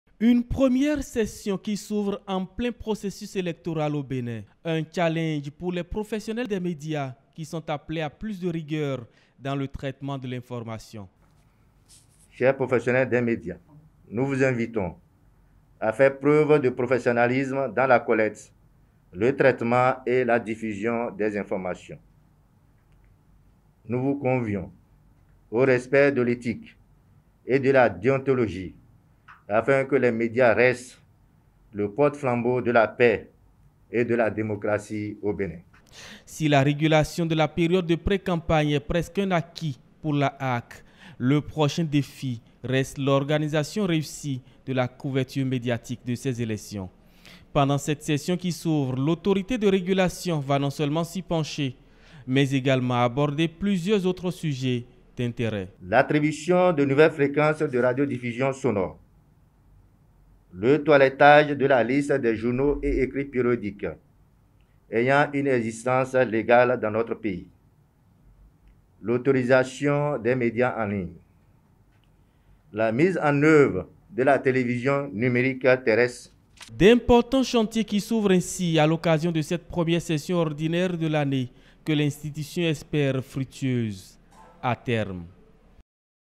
Médias : ouverture de la 1ère session ordinaire à la HAAC
Les travaux de la première session ordinaire de l'année 2021, à la Haute autorité de l'audiovisuel et de la communication ont démarré ce mardi. Les conseillers a la HAAC et les responsables d'associations professionnelles ont été conviés aux échanges.  William Aguiar et Igor Ayosso pour le compte rendu.